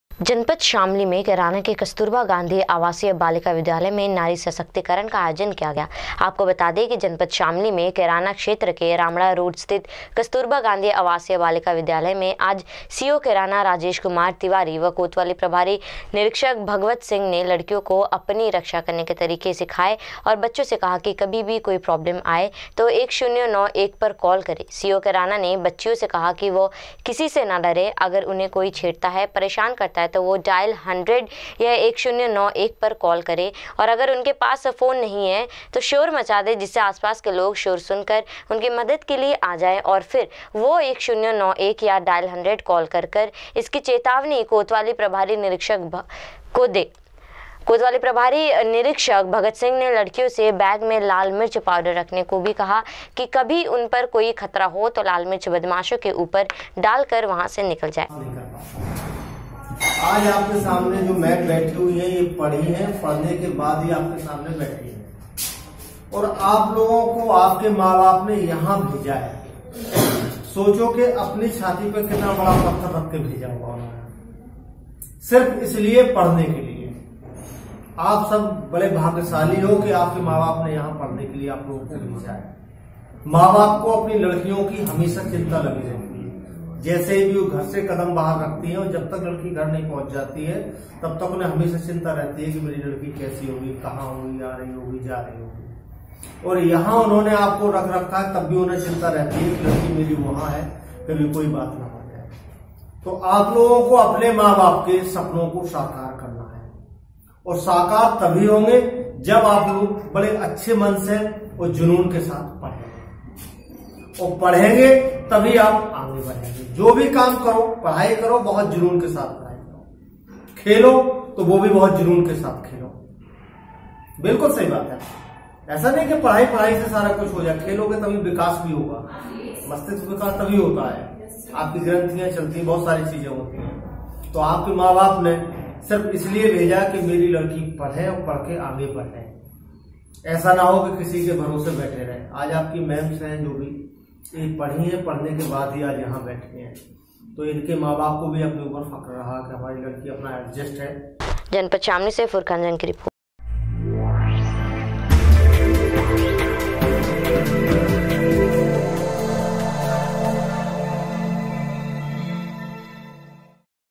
[Shamli News] Woman empowerment organized in Kasturba Gandhi residential girl school in Shamli
जनपद शामली मे कैराना के कस्तूरबा गांधी आवासीय बालिका विद्यालय में नारी सशक्तिकरण का आयोजन किया गया । आप को बतादे की जनपद शामली मे कैराना क्षेत्र के रामडा रोड स्थित कस्तूरबा गांधी आवासीय बालिका विद्यालय में आज सीओ कैराना राजेश कुमार तिवारी व कोतवाली प्रभारी निरीक्षक भगवत सिंह ने लड़कियों को अपनी रक्षा करने के तरीके सिखाए